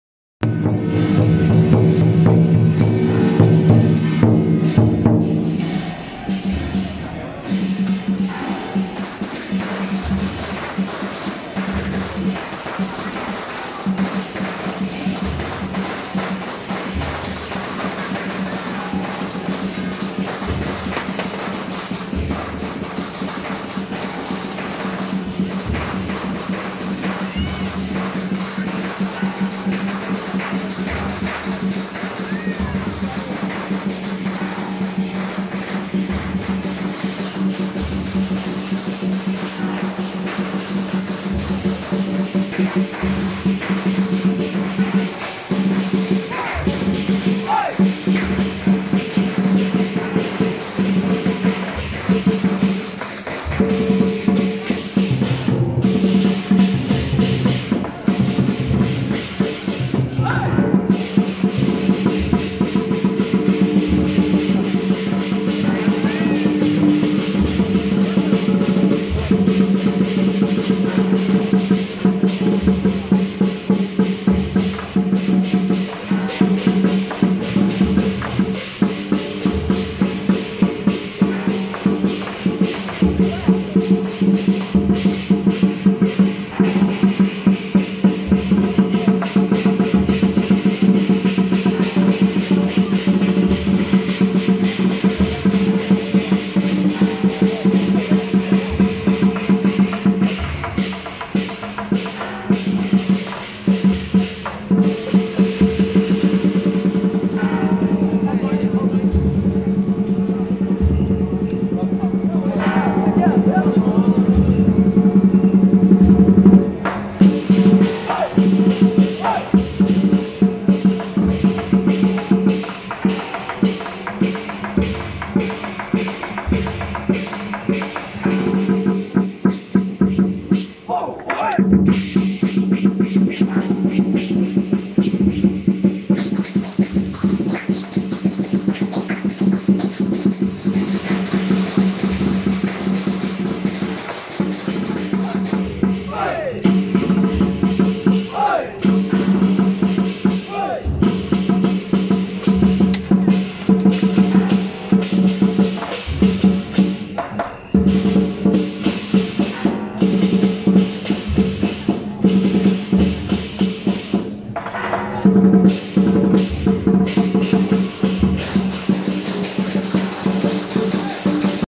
【台北祥薪舘-醒獅團】~109板橋無極北聖宮北極玄天上帝暨建宮20週年遶境
台北祥薪舘-醒獅團